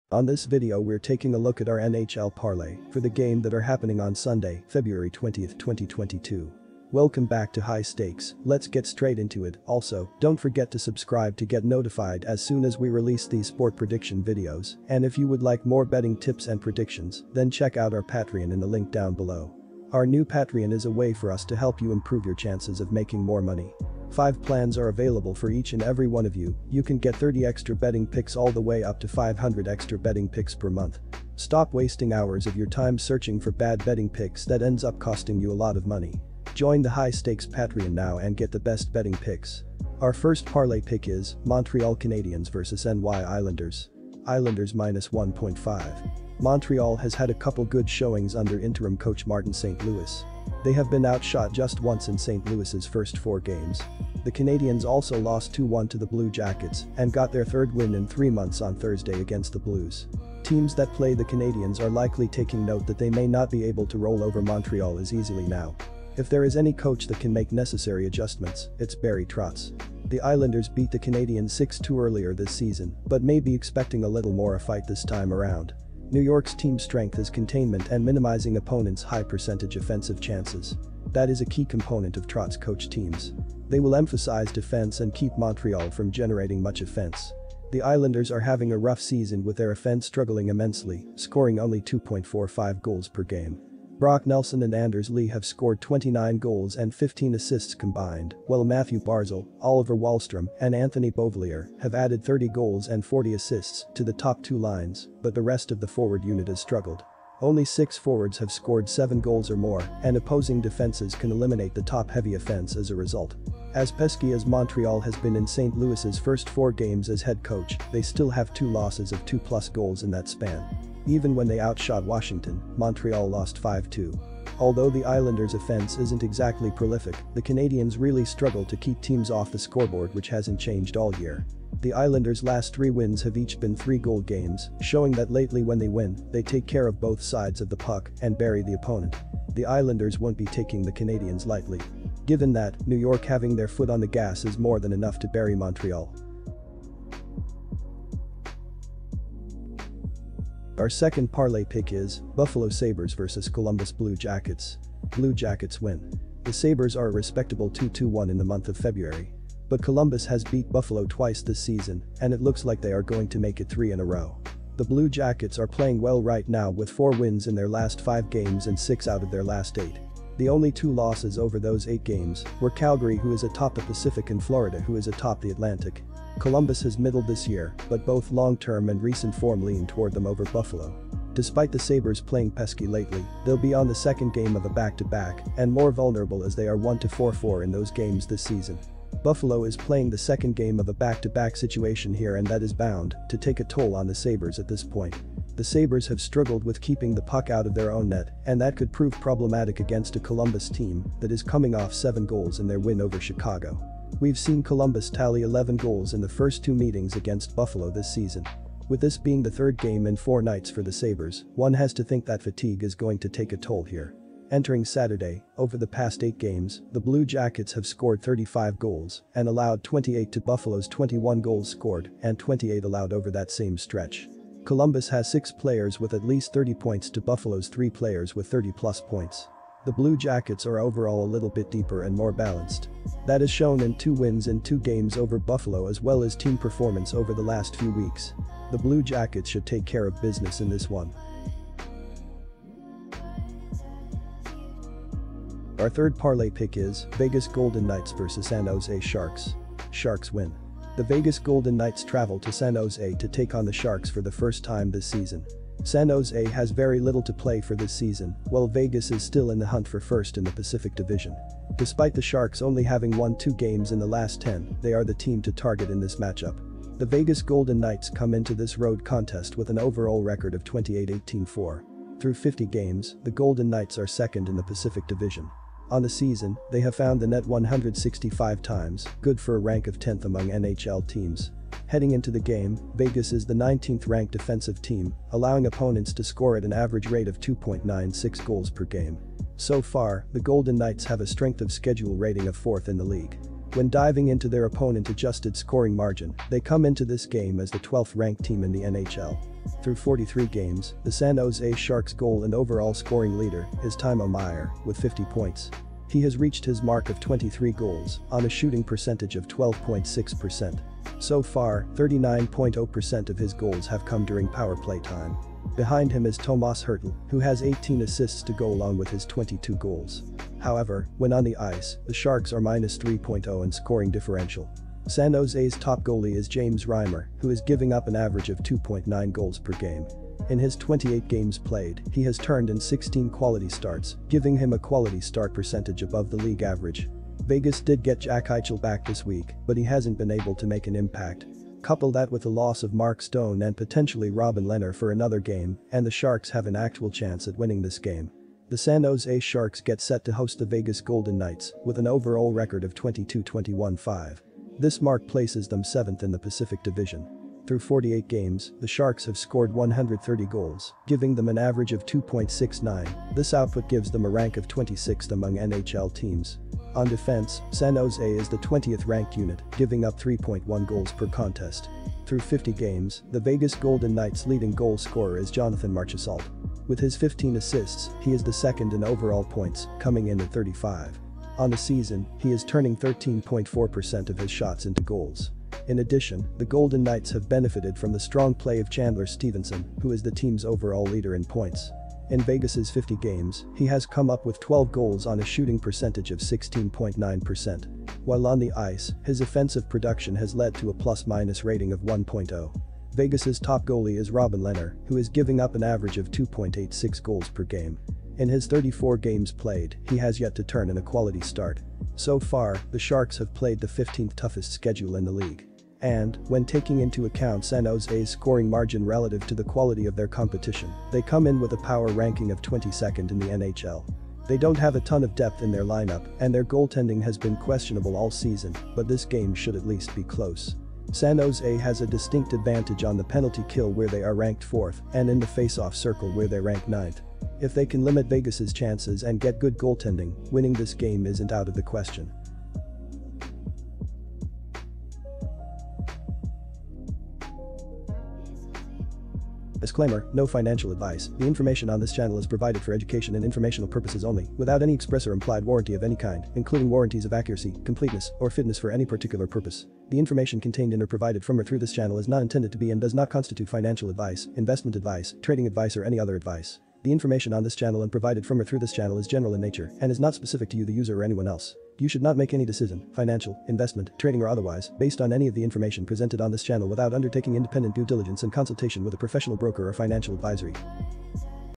FREE NHL 2/20/22 Parlay Picks and Predictions Today Over/Under NHL Betting Tips and Analysis
Montreal Canadiens vs N.Y. Islanders
Buffalo Sabres vs Columbus Blue Jackets
Vegas Golden Knights vs San Jose Sharks

Get 125% cashback bonus on your first deposit on the BetUS sportsbook after you open an account and deposit using our and use our promo code HIGHSTAKES when making your deposit.